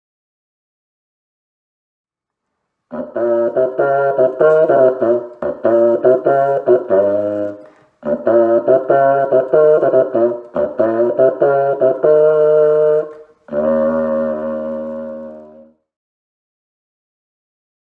Another flat-form bukkehorn bass
Another flat-form bukkehorn bass by Scott F. Hall in cut and laminated wood with a three-inch polyvinyl chloride tube bocal and a small-shank trombone mouthpiece. This original instrument design by Hall was shown along with six others by him at the Venice Architecture Biennale in August 2021 in the curated exhibition in the Italian Pavilion, FEEDback.